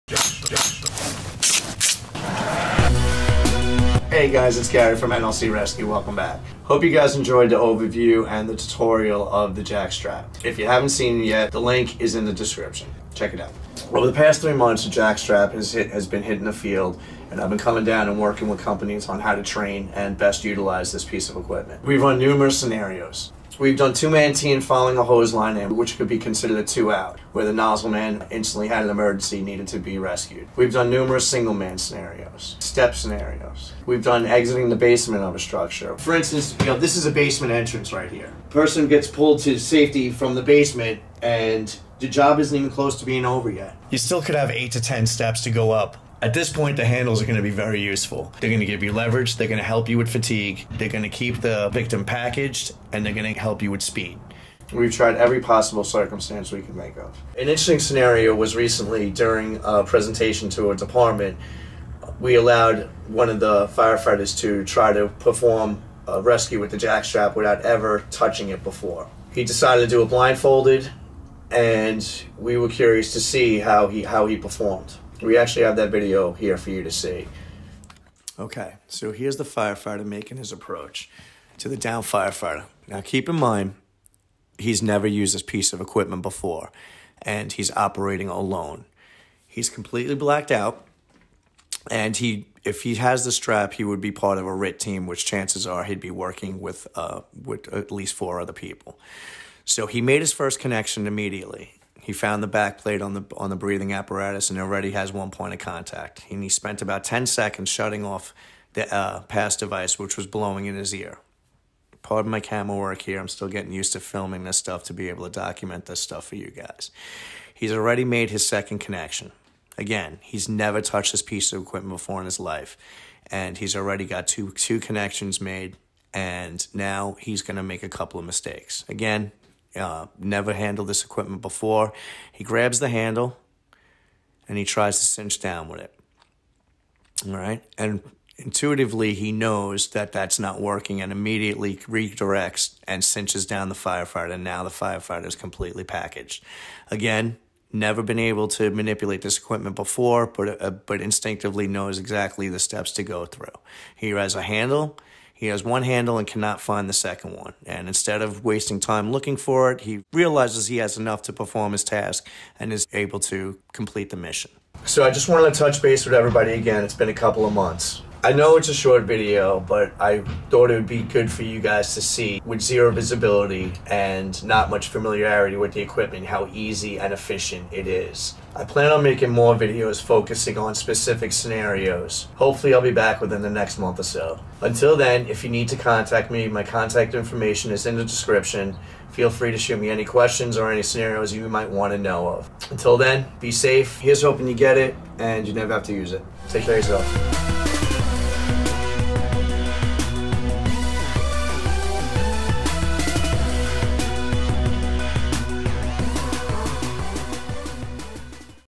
Jack Strap First Time Use
This is a video about our latest product from NLC Rescue, the "Jack Strap." A RIT tool used to rescue a down firefighter quickly and efficiently, find out how.

This video showcases a firefighter using the Jack Strap for the first time without and visibility nor instruction, let's see how he does...